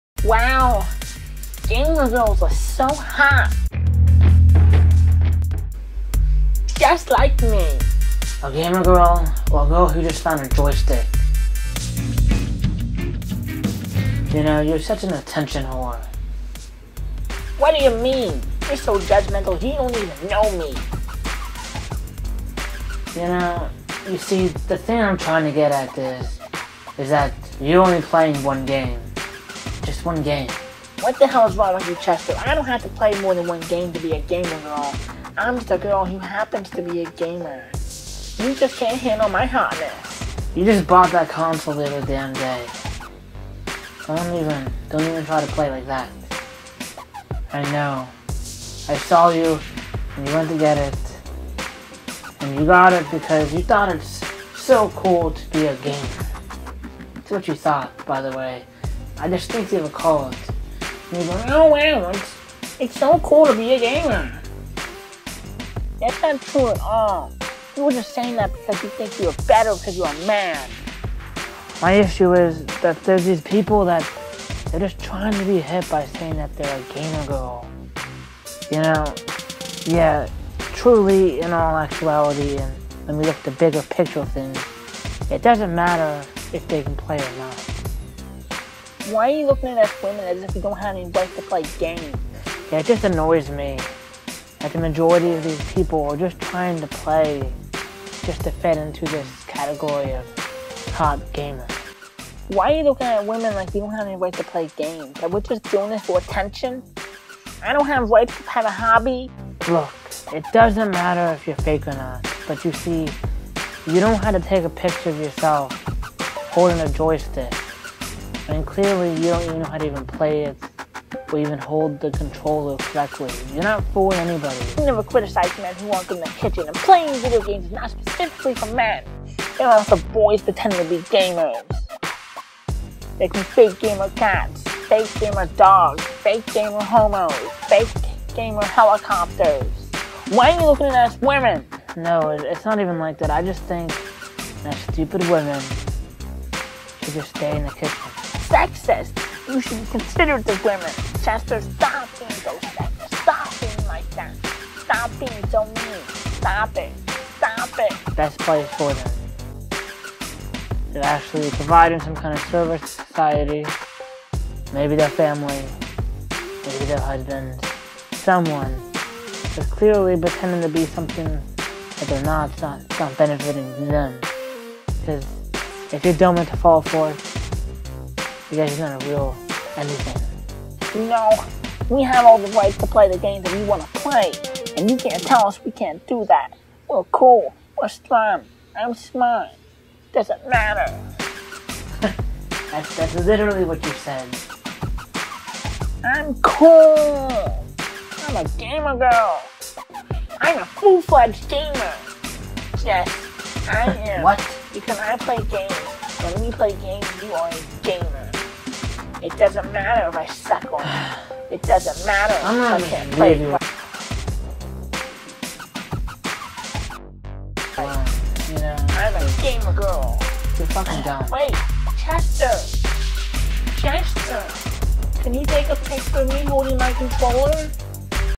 GIRLS CAN'T BE GAMERS!!
You know what pisses me off? People who are fucking fake--there is this shit that has been going on with ladies for awhile pretending to be gamers for the attention that they so greatly crave. Fuck fake bitches.

Links:
Twitter.com/ImAtTheBridge
Facebook.com/IamAtTheBridge